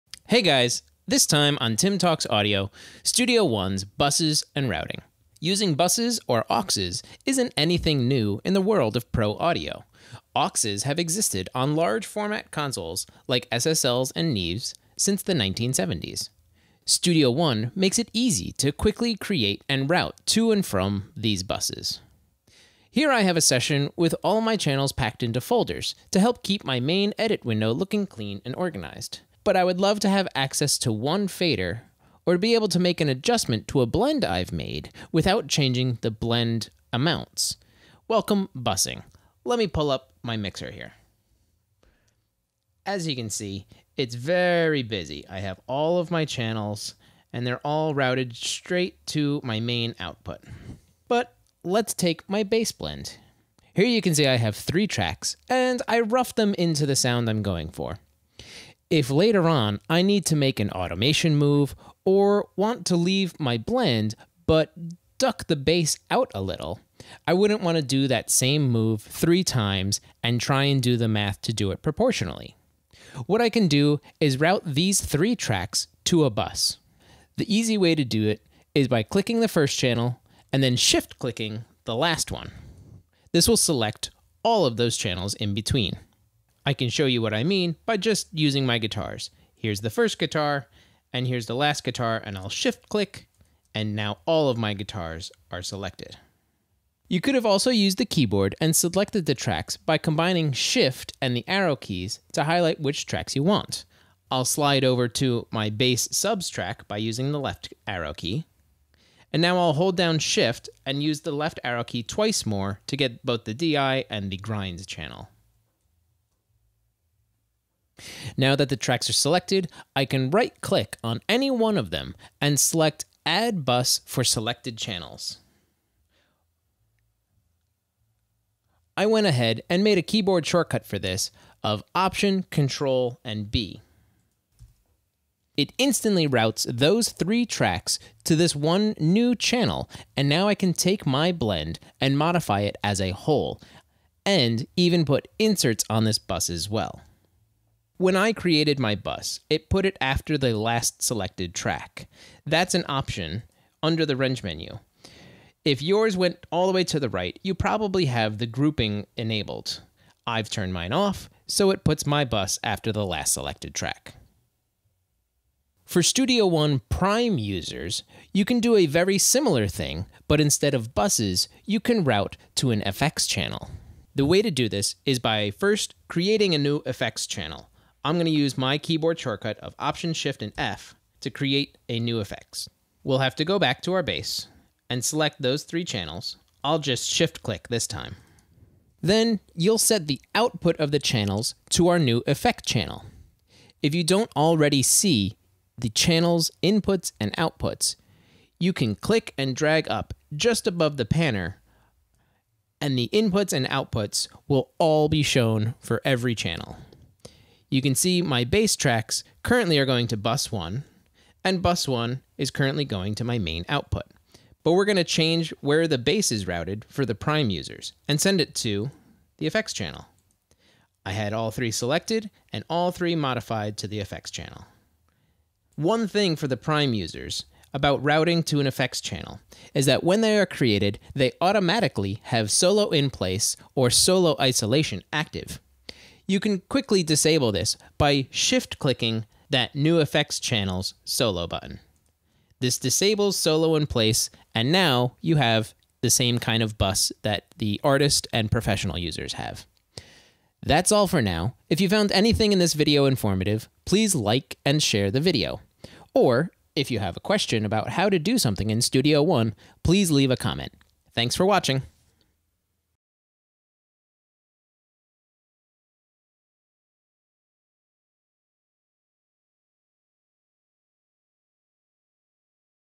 Studio One - Buses and Routing
Everyone on the bus? Great! Grand! Wonderful! This video shows you how to quick;y and easily set up buses in studio one for users with Professional, Artist and Prime users!

Have a question about Presonus Studio One? Ask in the comments and I'll make your question into a video for everyone else to learn from!